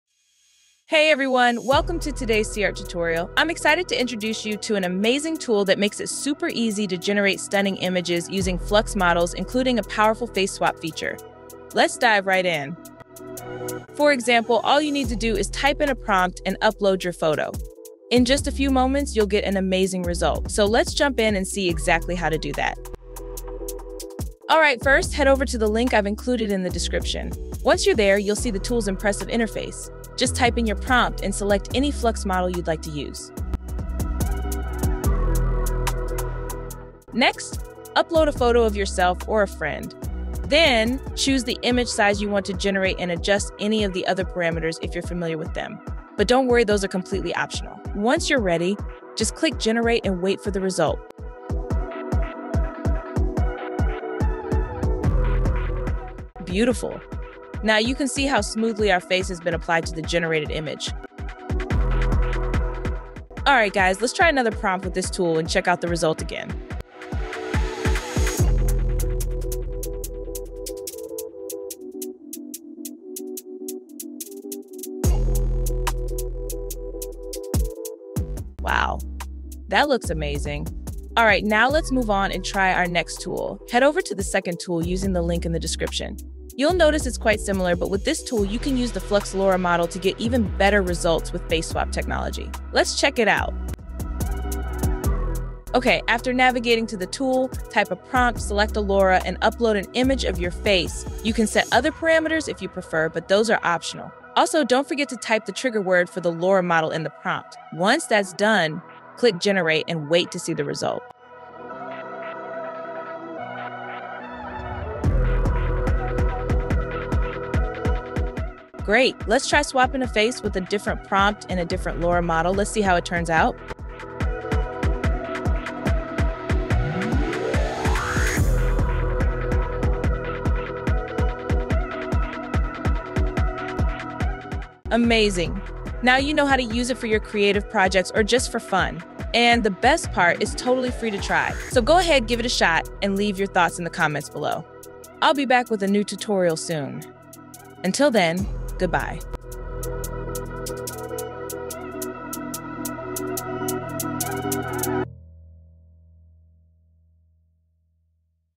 Generate Realistic Images Including Your Face in 1 Minute! (Advanced Face Swap with Flux Models)
Learn how to quickly generate realistic face swaps using SeaArt's Flux models in just under a minute with this easy-to-follow tutorial!